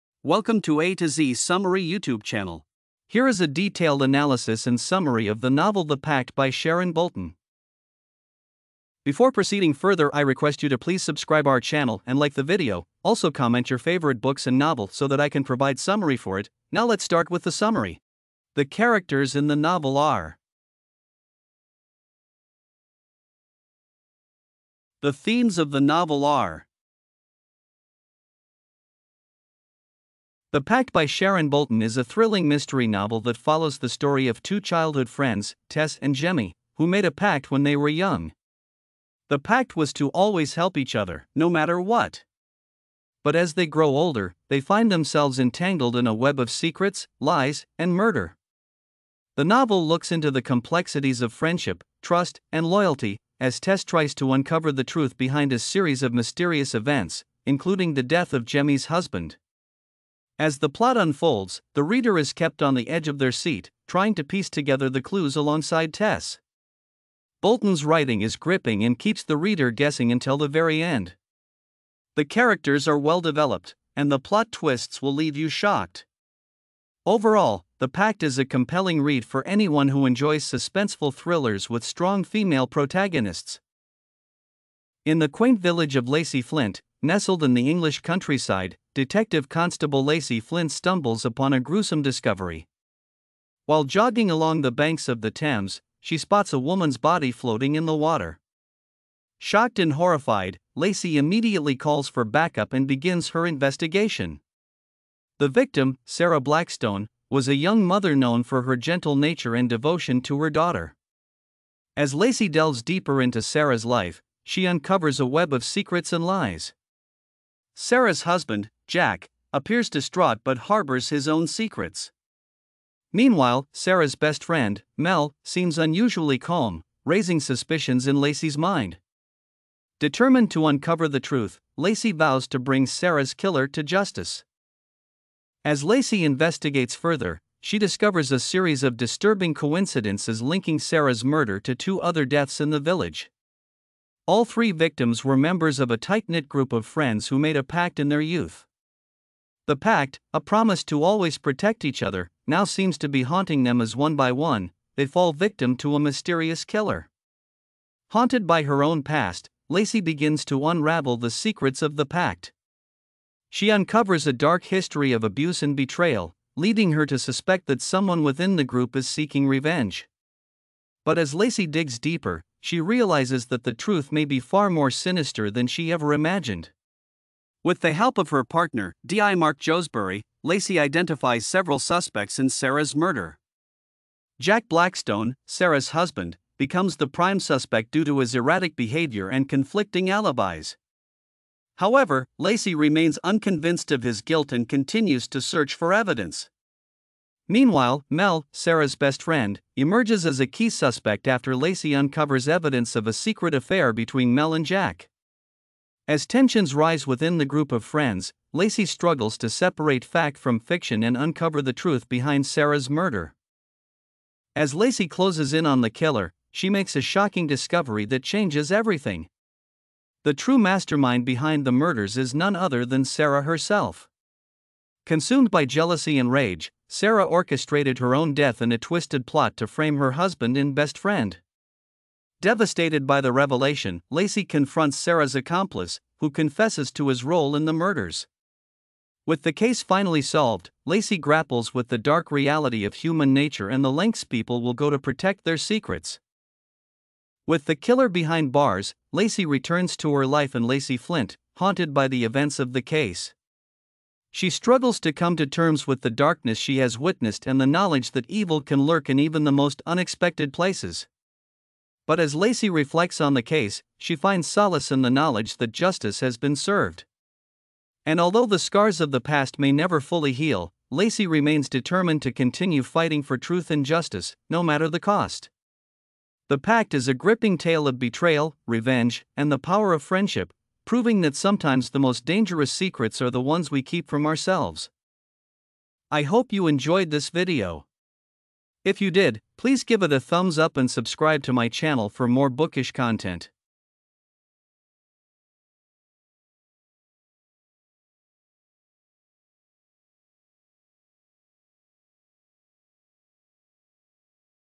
"The Pact" by Sharon Bolton | Summary, Themes, Characters & Analysis (Audiobook)
Dive into the thrilling audiobook, "The Pact" by Sharon Bolton!
Unravel a dark mystery ️‍♀️ as two families become entangled in a web of deceit ️

***Summary***
Two families. One deadly secret. Lexi, a young lawyer, is determined to uncover the truth behind her niece's disappearance. But as she digs deeper, she unearths a shocking connection to a wealthy family and a past tragedy. Meanwhile, DI Mark Hobson investigates a seemingly unrelated case, only to find his path colliding with Lexi's. As their investigation intensifies, they realize a dangerous pact binds the two families, and the truth could be deadlier than they ever imagined. ⚠️

***Themes***
- Deception and betrayal
- Secrets and lies
- Family bonds (both strong and twisted) ‍‍‍
- The pursuit of justice ⚖️

***Characters***
- Lexi: A tenacious lawyer on a mission for the truth.
- DI Mark Hobson: A seasoned detective with a keen eye for detail.
- Prepare to be surprised by a cast of characters with hidden agendas and dark secrets.

***Analysis***
This gripping audiobook will keep you guessing until the very end. Bolton masterfully weaves together suspense, intrigue, and psychological depth, making "The Pact" a must-listen for fans of crime thrillers.